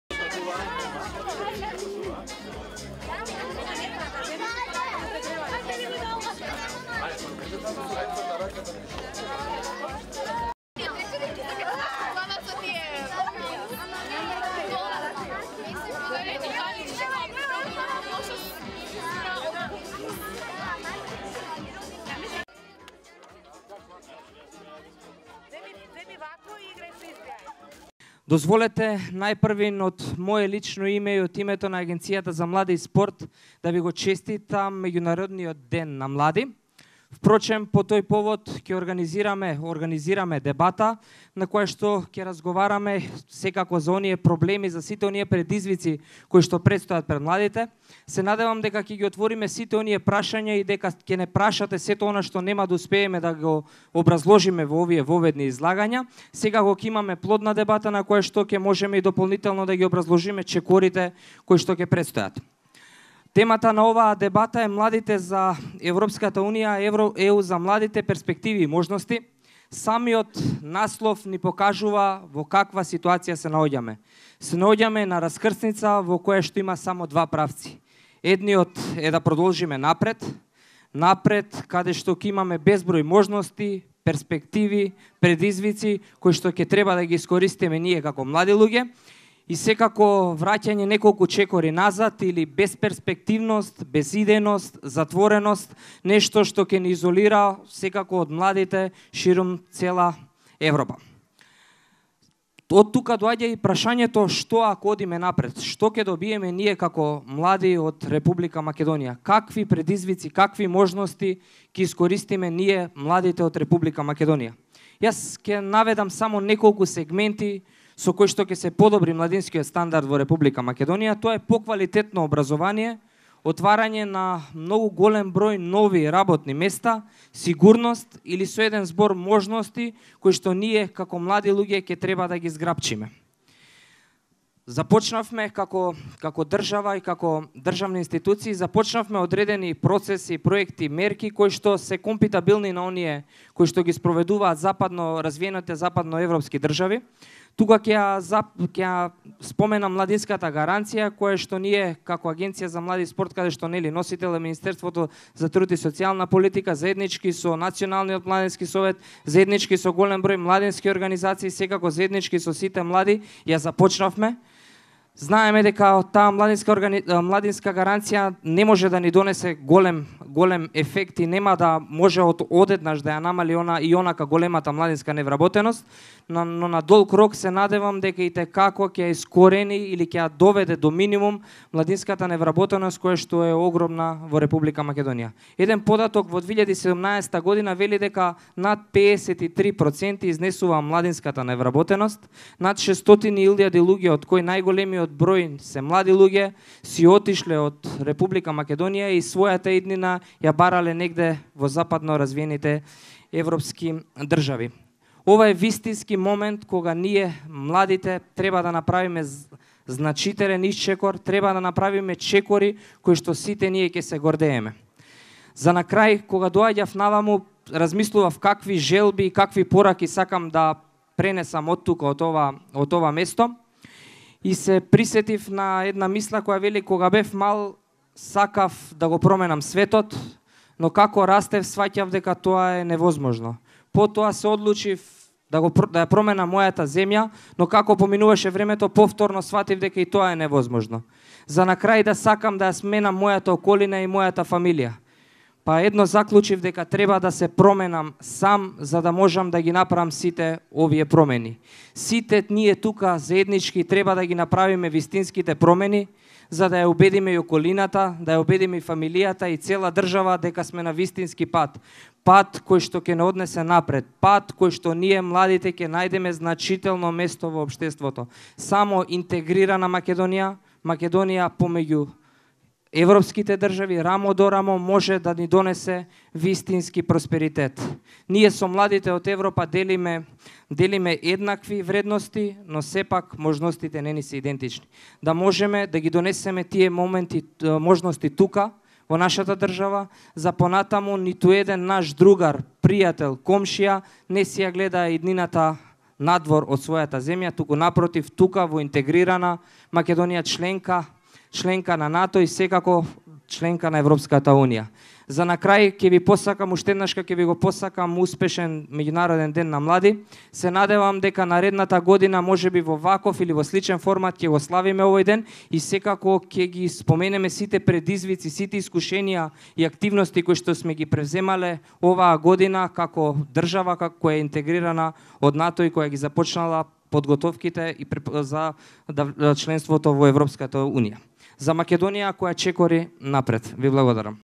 АМС Каевски: Младите да градат иднина во Македонија која е дел од НАТО и ЕУ
vlada.mk/node/15372